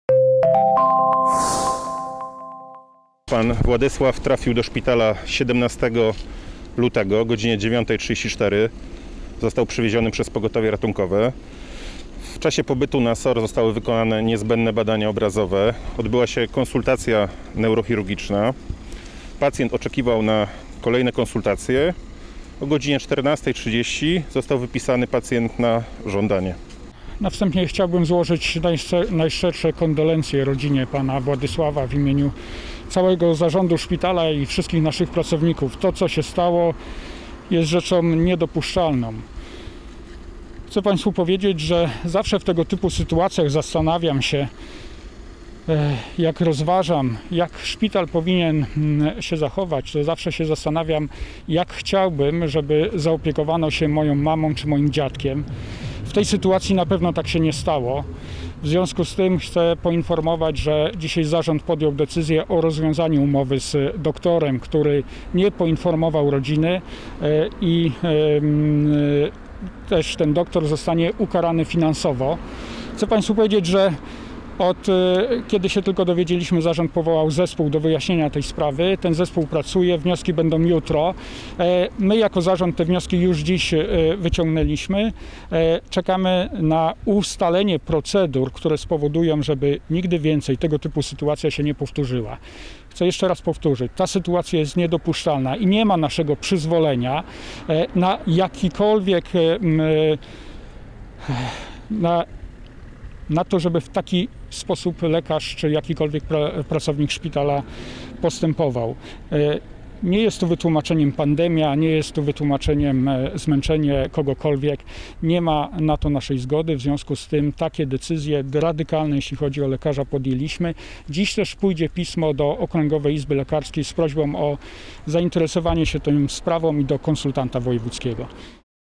Gorzowski szpital wyjaśnia sprawę zaginięcia i śmierci pacjenta
W minioną środę chorego przywiozła na gorzowski SOR karetka. Po badaniach wypisany został - jak wyjaśnia szpital - na własne żądanie. Po opuszczeniu lecznicy zgłoszono zaginięcie mężczyzny.  Finał poszukiwań niestety okazał się tragiczny.
W celu wyjaśnienia sprawy zarząd Wielospecjalistycznego Szpitala Wojewódzkiego w Gorzowie powołał czteroosobowy zespół. Wiceprezes Robert Surowiec na specjalnie zwołanej konferencji prasowej poinformował dziś, że szpital w trybie natychmiastowym rozwiązał umowę z lekarzem, który wypuścił pacjenta z oddziału.